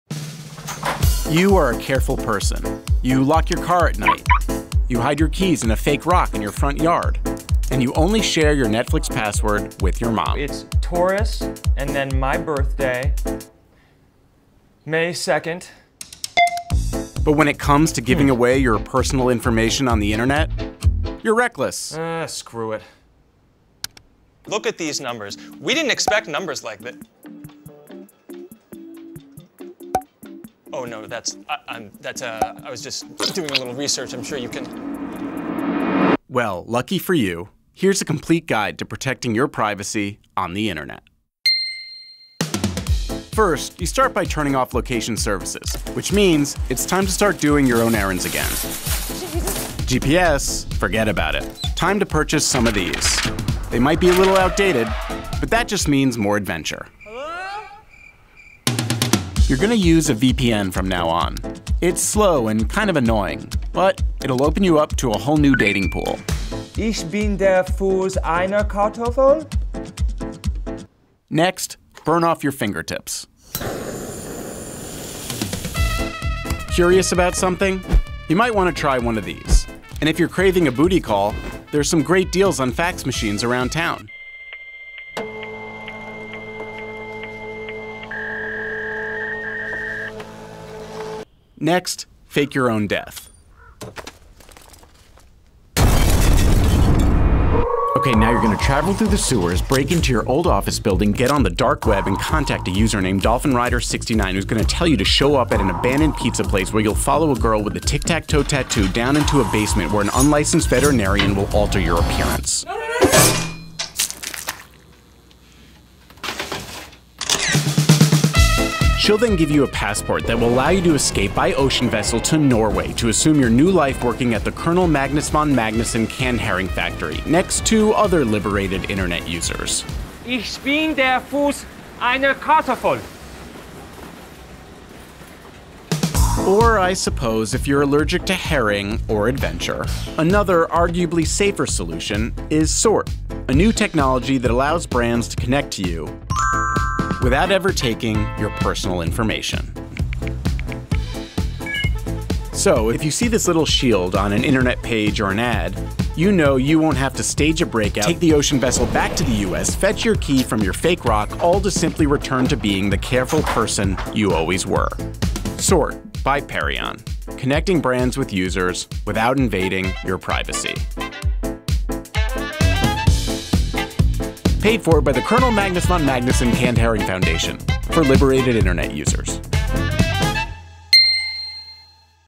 A Complete Guide to Protecting Your Privacy on the Internet
A couple weeks ago, an ad tech company called Perion reached out to me to see if I'd make a video for a new product of theirs. So I got a couple of friends together and we made this epic video - a spiritual sequel to Follow The Frog.

Here is Perion's website where you can read more about the SORT technology